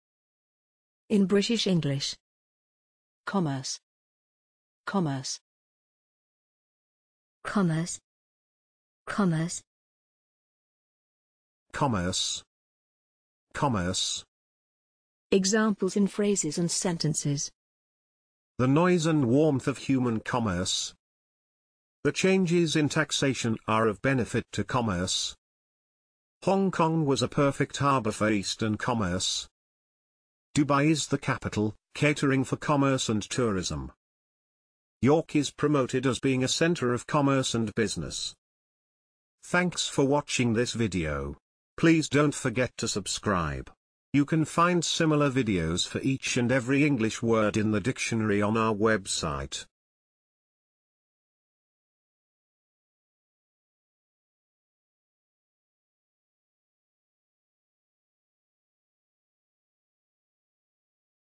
commerce - pronunciation + Examples in sentences and phrases
This is the pronunciation of commerce in British English + a few examples of its pronunciation in phrases and sentences.